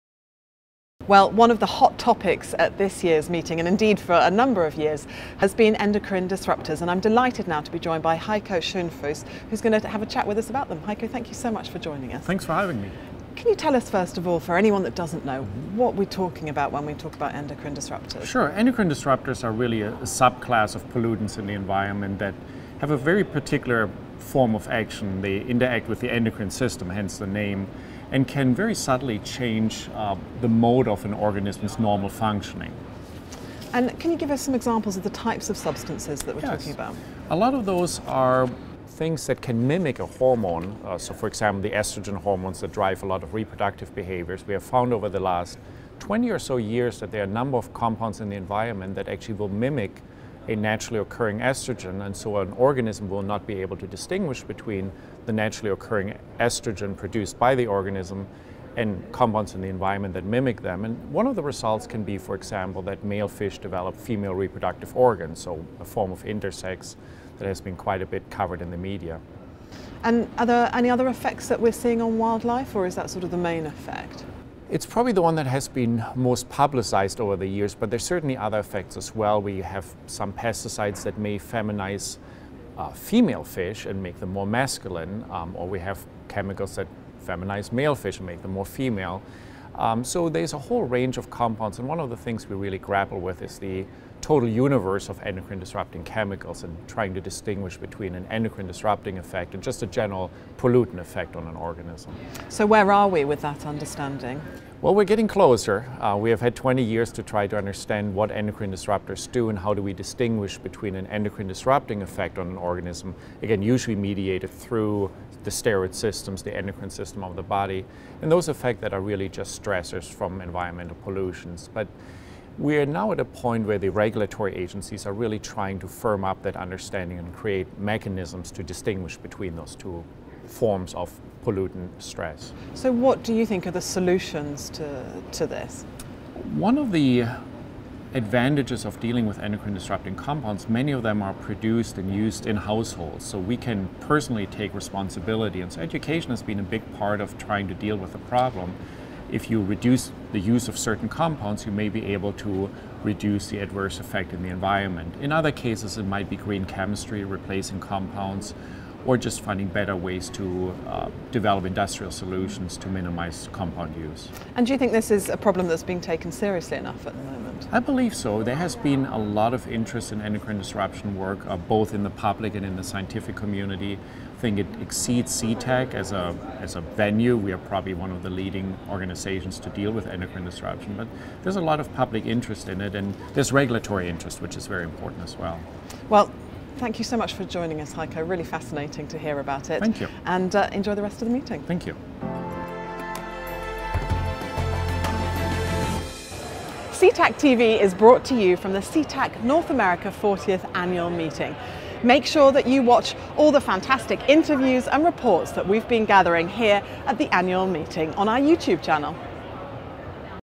Endocrine Disruptors with Heiko Schoenfuss - SETAC 2019
Endocrine Disruptors is one of the hot topics at this year’s SETAC North America 40th Annual Meeting.  Heiko Schoenfuss, Co-Chair of the Endocrine Disrupter Testing and Risk Assessment Interest Group dropped into the SETAC TV studio to bring us up to date on the latest in this field.